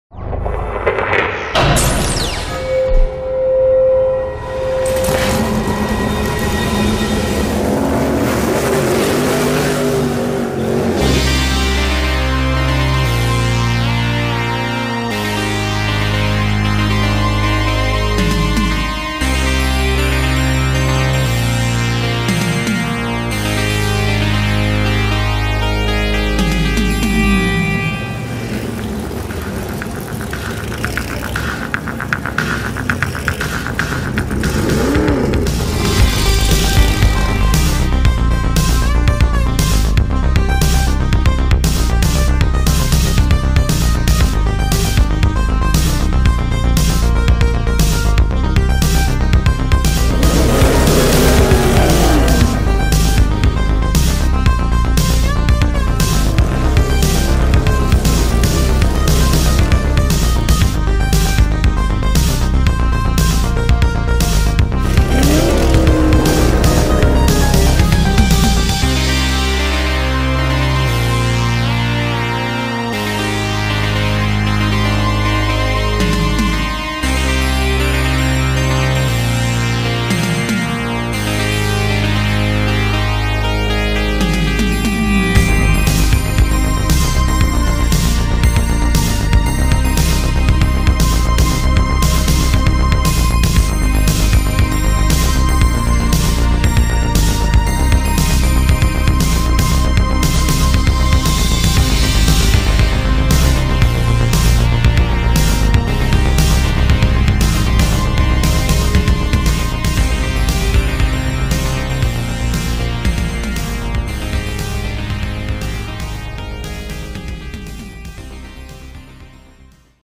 STOCK CAR 2018 - UMA SANTA CRUZ DO SUL RETRÔ!
Com direito a um visual retrô e lotado de néons, relembramos a etapa de Santa Cruz do Sul vencida por Marcos Gomes e Átila Abreu!